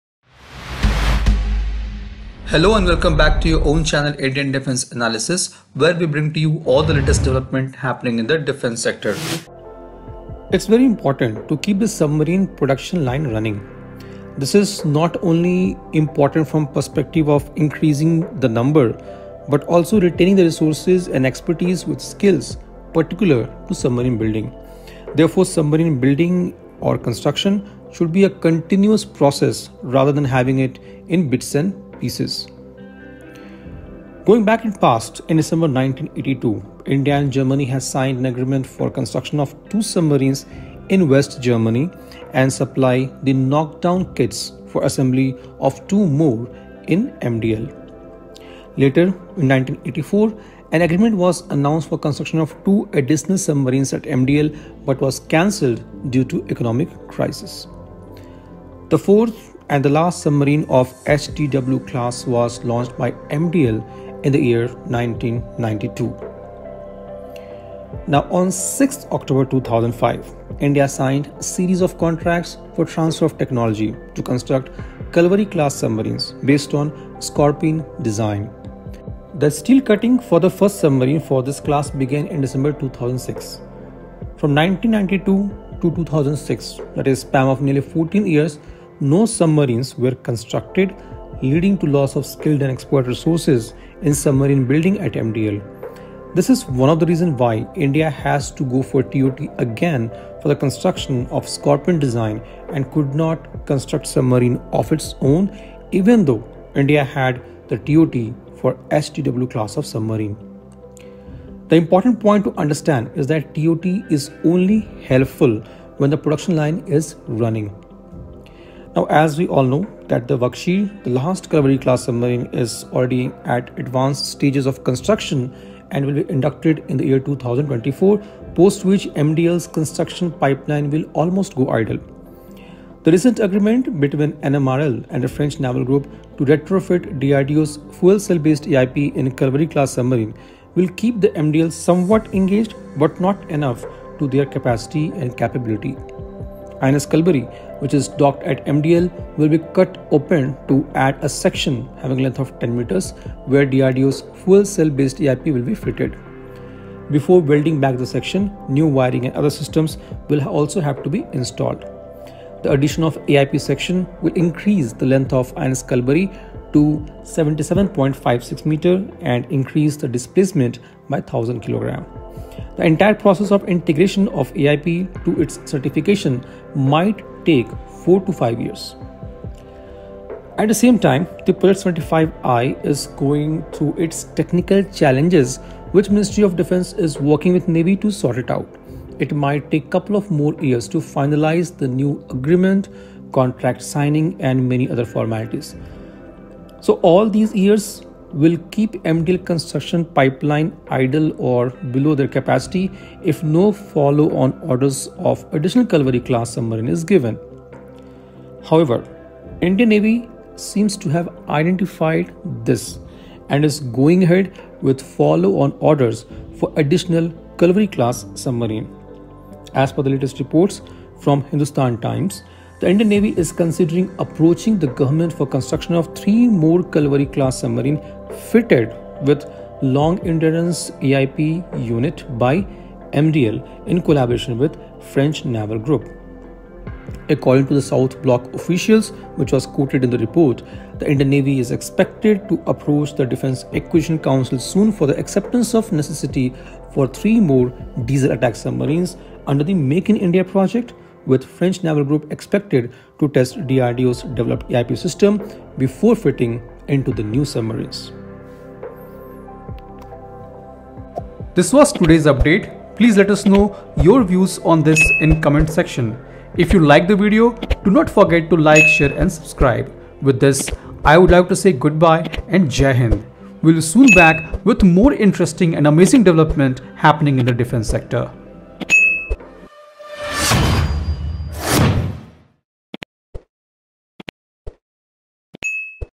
Navy to approach DAC for AON for 3 Kalvari class Diesel Electric Submarine equipped with DRDO's AIP
Submarine building must be continuous

It’s important to keep the submarine production line running. This is not only important from perspective of increasing number but also retaining the resources and expertise with skills particular to submarine building. Therefore submarine building/ construction should be continuous process rather than having it in bits in pieces.

4 HDW Class submarine deal

In December 1982, India and Germany has signed an agreement for the construction of 2 submarines in West Germany and supply of knocked down kits for assembly of 2 or more in MDL. later in 1984 an agreement was announced for construction of 2 additional submarines at MDL but was cancelled due to the economic crisis. The 4th and last submarine of HDW class was launched by MDL in 1992.

6 Kalvari Class submarine deal

On 6th October 2005, India signed a series of contracts for ToT to construct Kalvari class submarines based on Scorpene class design. The steel cutting for the 1st submarine of this class began in December 2008.

From 1992 to 2006 i.e. a spam of nearly 14 years no submarine were constructed, leading to loss of skilled & expert resources in submarine building at MDL. This is one of the reason why India has to go for ToT again for the construction of Scorpene design and could not construct submarine of its own, even though India had ToT for HDW class submarine. The important point to understand is that, ToT is only helpful when the production line is running.

MDL construction line to almost go idle

As we all know that Vagsheer, the last Kalvari class submarine is already at advance stage of construction and will be inducted in 2024, post which MDL’s construction pipeline will almost go idle.

The recent agreement between NMRL & French Naval group to retrofit the DRDO’s Fuel cell based AIP in Kalvari class submarine will keep the MDL somewhat engaged but not enough to their capacity & capability.

At the same time Project-75I is going through its technical challenges which MoD is working with Navy to sort out. It might take couple of more years to finalize the new agreement, contract signing and other formalities. All these years will keep MDL construction pipeline idle if no follow on orders of additional Kalvari class submarine is not given.

3 more Kalvari Class submarine with DRDO’s AIP

Indian Navy seems to have identified this and is going ahead with follow on orders for additional Kalvari class submarines.

As per reports from Hindustan Times report, The Indian Navy is considering approaching the government for the construction of three more Kalvari class submarines, fitted with long endurance air independent propulsion (AIP) unit, by Mazagon Dock Shipbuilders Ltd (MDL) in collaboration with French Naval Group. According to South Block officials, the Indian Navy is expected to approach the Defence Acquisition Council (DAC) soon for acceptance of necessity (AoN) for three more diesel attack submarines under the “Make in India” project with the French Naval Group expected to test the DRDO developed AIP system before fitting in the new submarines.